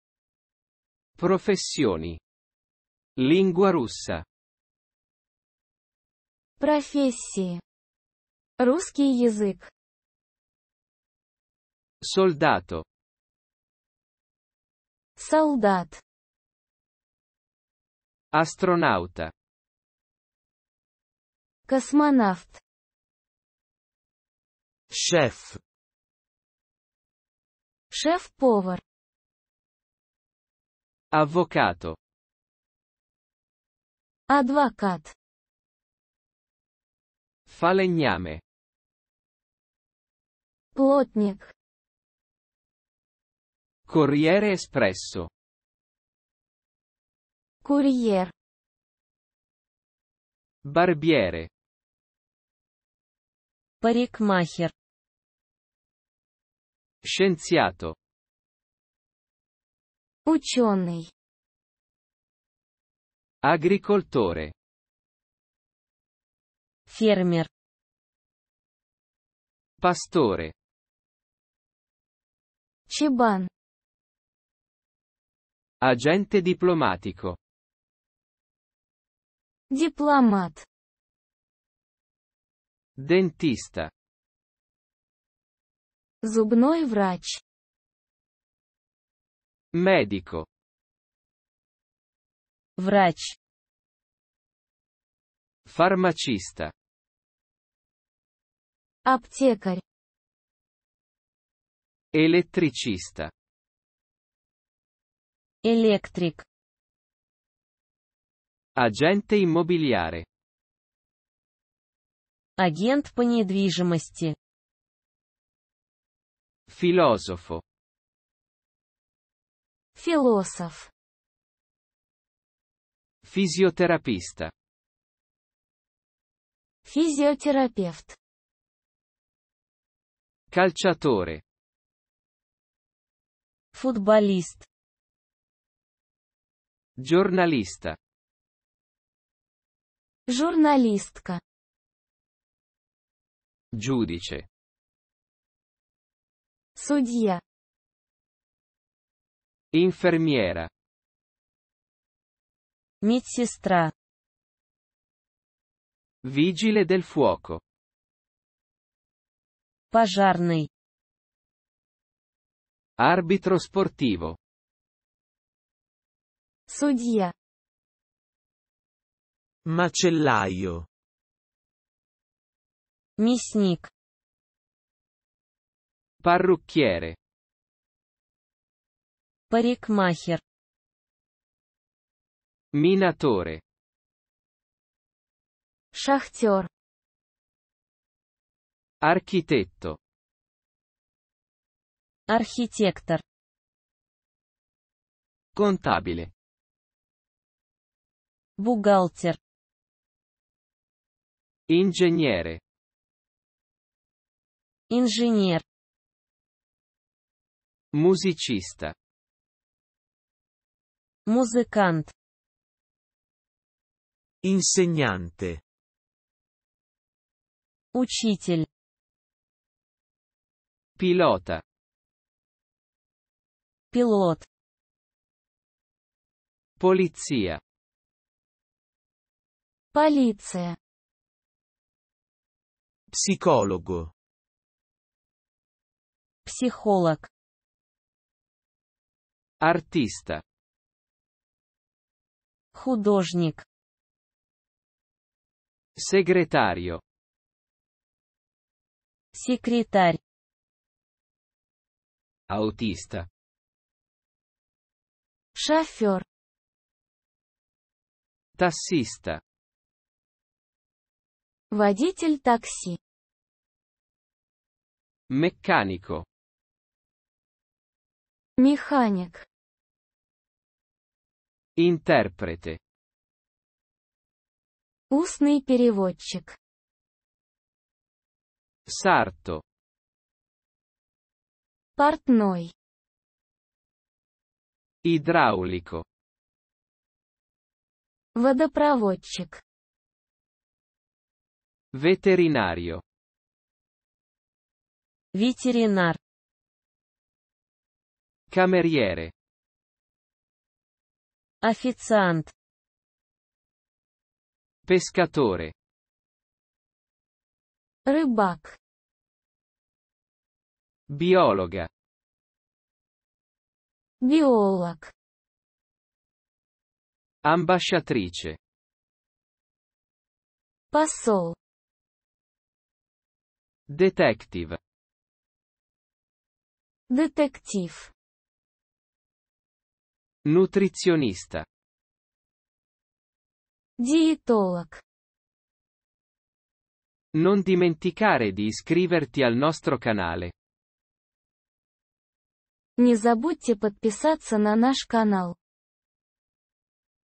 Professioni (Lingua russa) (it-ru)
Fone TV
it-ru-7
Профессии (Русский язык)
Soldato - Солдат
Astronauta - Космонавт
Chef - шеф-повар
Avvocato - Адвокат
Falegname - Плотник
Corriere espresso - Курьер
Barbiere - Парикмахер
Scienziato - Ученый
Agricoltore - Фермер
Pastore - Чабан
Agente diplomatico - Дипломат
Dentista - Зубной врач
Medico - Врач
Farmacista - Аптекарь
Elettricista - Электрик
Agente immobiliare - Агент по недвижимости
Filosofo - Философ
Fisioterapista - Физиотерапевт
Calciatore - Футболист
Giornalista - Журналистка
Giudice - Судья
Infermiera - Медсестра
Vigile del fuoco - Пожарный
Arbitro sportivo - Судья
Macellaio - Мясник
Parrucchiere - Парикмахер
Minatore - Шахтёр
Architetto - Архитектор
Contabile - Бухгалтер
Ingegnere - Инженер
Musicista - Музыкант
Insegnante - Учитель
Pilota - Пилот
Polizia - Полиция
Psicologo - Психолог
Artista - Художник
Segretario - Секретарь
Autista - Шофер
Tassista - Водитель такси
Meccanico - Механик
Interprete - Устный переводчик
Sarto - Портной
Idraulico - Водопроводчик
Veterinario - Ветеринар
Cameriere - Официант
Pescatore - Рыбак
Biologa - Биолог
Ambasciatrice - Посол
Detective - Детектив
Nutrizionista - Диетолог